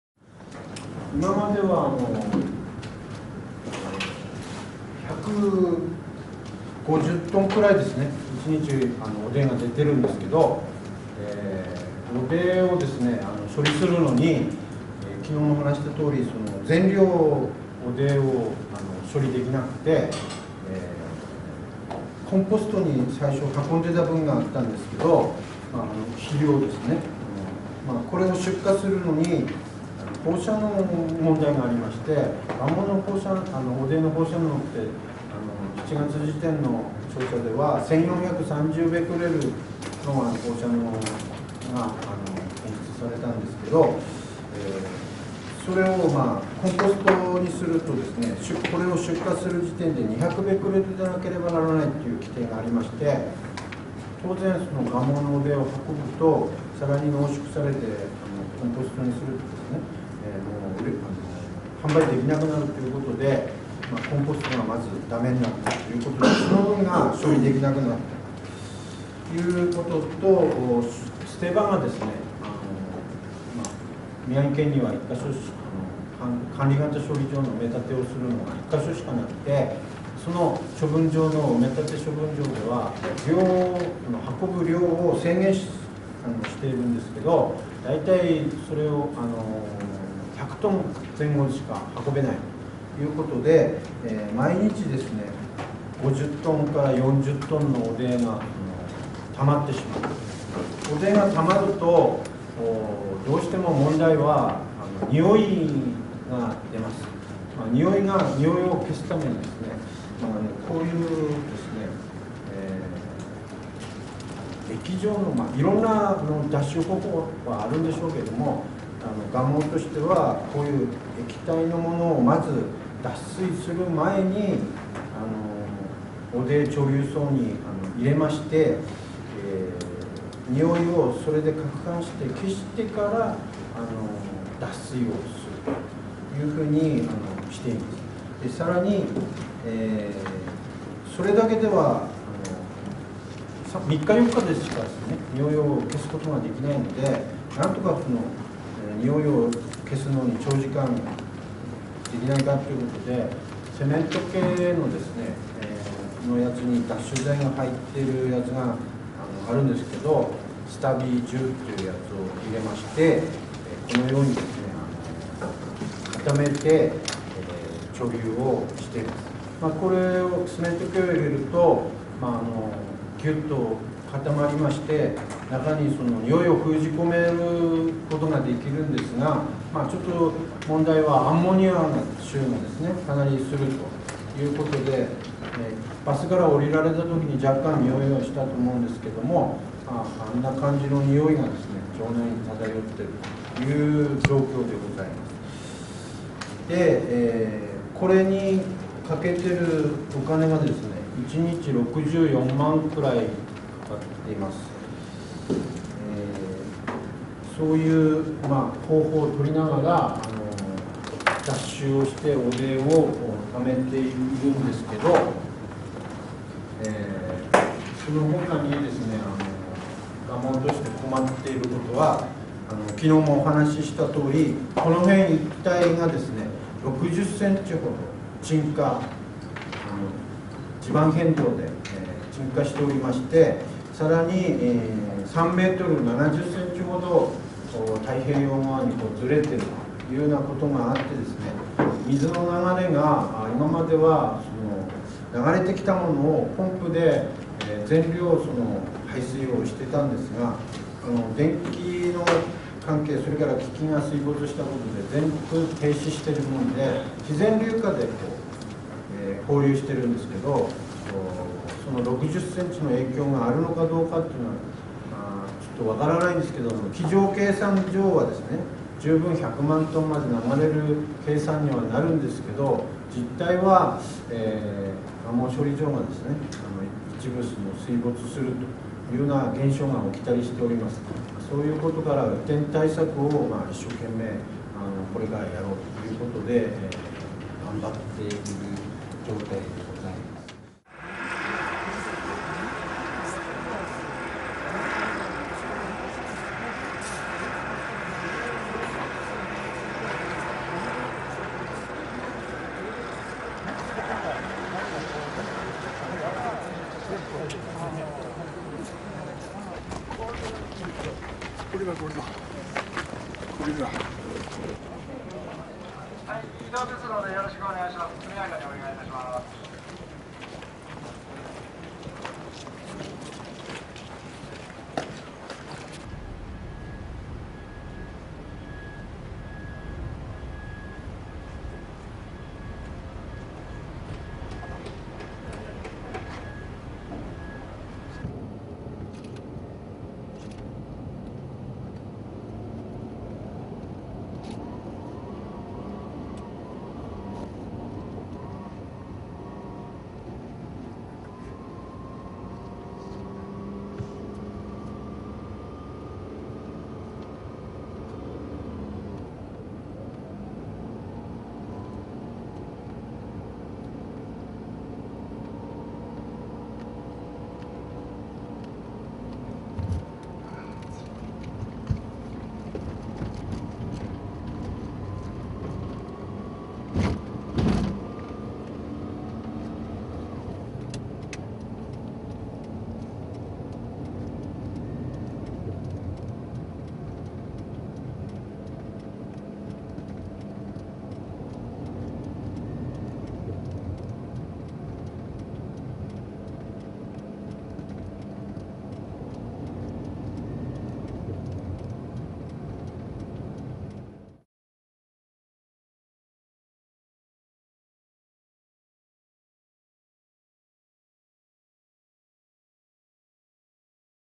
Record of Minamigamo Sewage Plant / 南蒲生浄化センターの記録
The condition of Minamigamo Sewage Plant, which processes about 70% of Sendai City's wastewater, was explained at an on-site study meeting, "The 14th Japan Society on Water Environment Symposium." The plant is currently engaged in treatment of dehydrated sludge, and is struggling with problems such as enriched radioactive materials and damaged equipment.

On-site study meeting of Japan Society on Water Environment: Photographs and articles of Gamo Tidal Flat and Minamigamo Sewage Treatment Plant

Filmed by Yoshitomo Nagasaki (recorder311), November 11, 2011

仙台市の汚水の約7割を処理する南蒲生浄化センターの様子を、「第14回 日本水環境学会シンポジウム」の現地見学会で伺いました。 脱水後の汚泥の処理は、津波による設備の被害と放射性物質の濃縮という問題を抱えた状況で行っています。
日本水環境学会現地見学会：蒲生干潟と南蒲生浄化センターのしゃしんときじ

撮影者：長崎由幹（ながさき　よしとも）：recorder311

３がつ１１にちをわすれないためにセンター